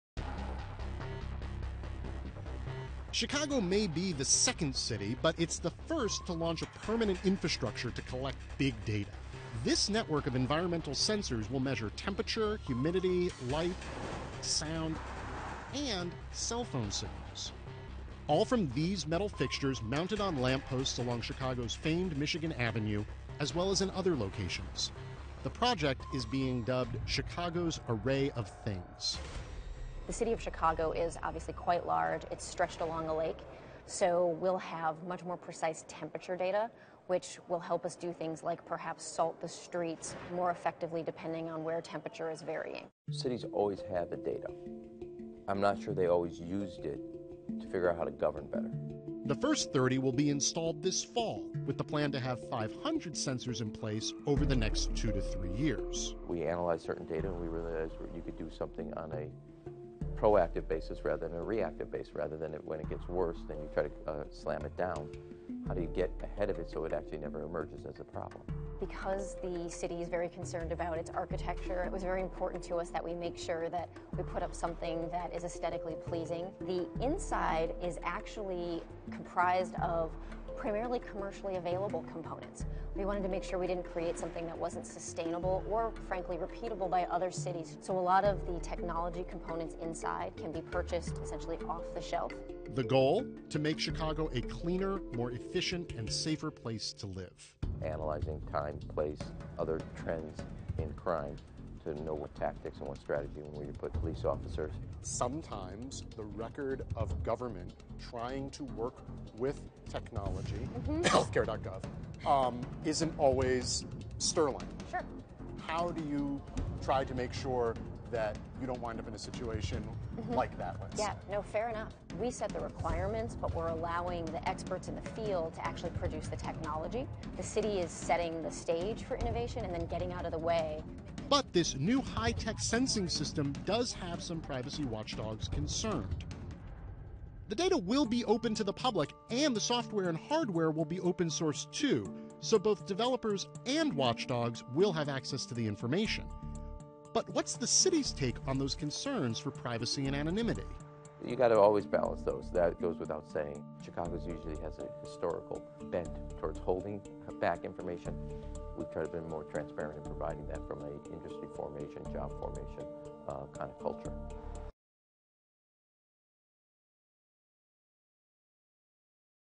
Big Data: Chicago Is Putting Sensors All Over the City
Aug. 27 (Bloomberg) -- The city of Chicago is betting big on big data. Mayor Rahm Emanuel has given the green light to a project to install sensors on traffic poles that collect data on air quality and weather. Now some privacy groups concerned. Bloomberg's Sam Grobart explains. (Source: Bloomberg)